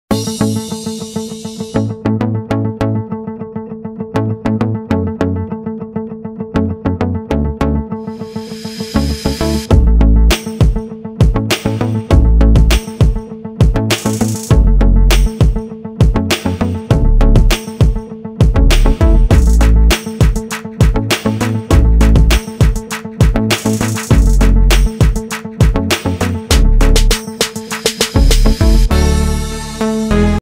Rohová vana levá 169 x 80 cm bílá GOCTA
Luxusní vana v nádherném elegantním designu. Povrch je vroubkovaný, což dodává na netradičním vzhledu. Umístěte ji do rohu vaší koupelny a doplňte stylovou baterií. Rozměry: 59 cm výška, 75 cm šířka, 170 cm délka, objem 280 l, nosnost 190 kg. Materiál: akryl